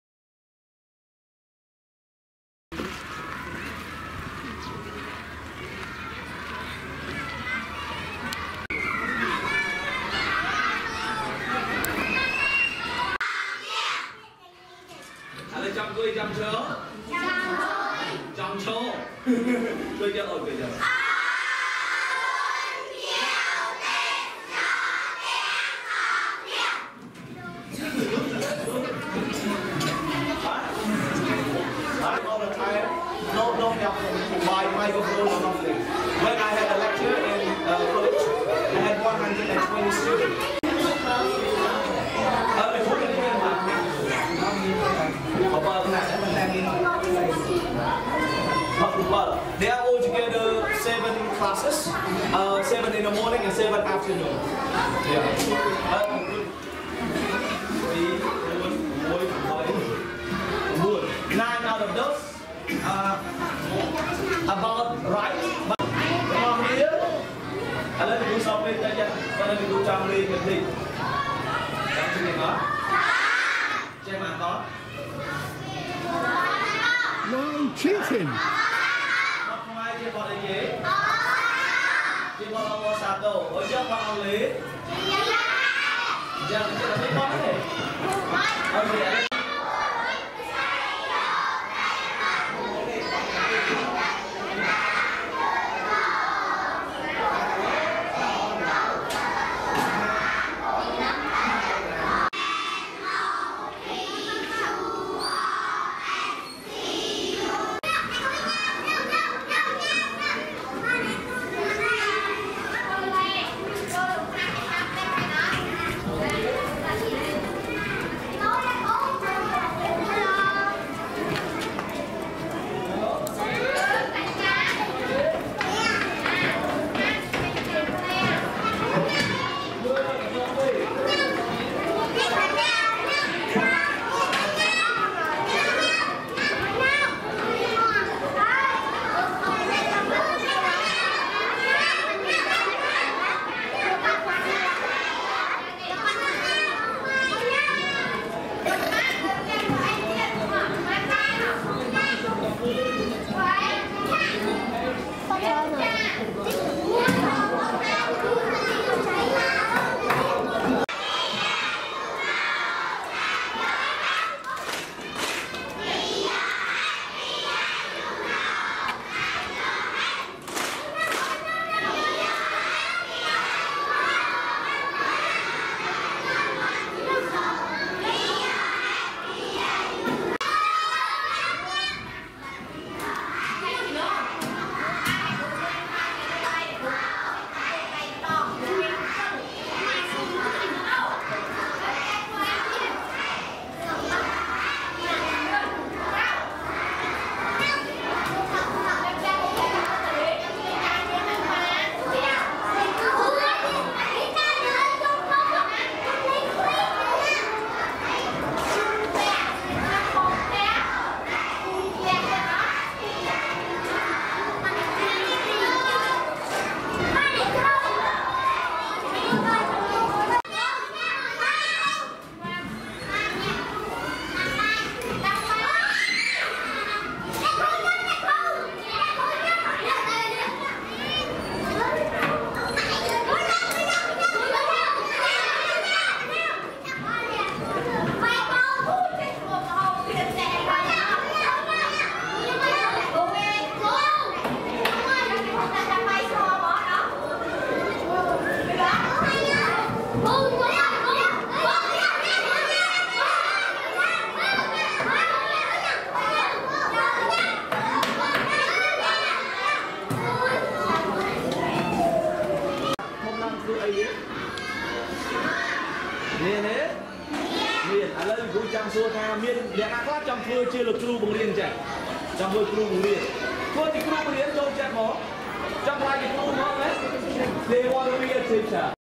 School class in Koh Chen, Cambodia
On our tour through Cambodia we visited an elementary school in Koh Chen on the Tonle Sap River.